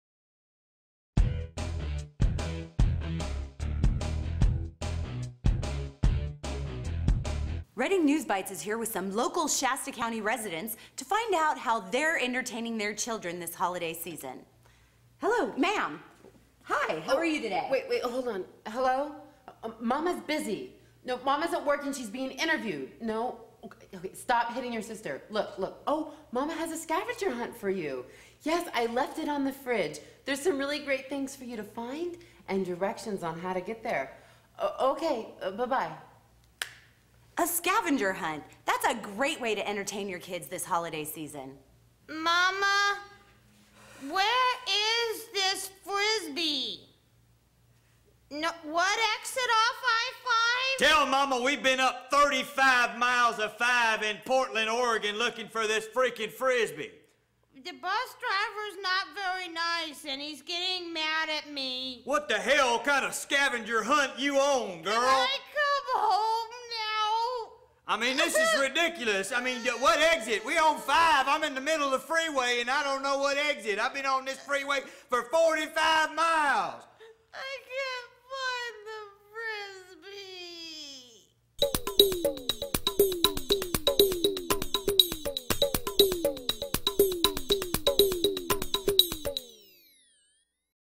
Keeping the kids busy
Christmas vacation for most students starts at the end of the week, and if you're the parent of young children, you know keeping them busy for the next two weeks will tap every ounce of energy, intelligence and creativity you possess.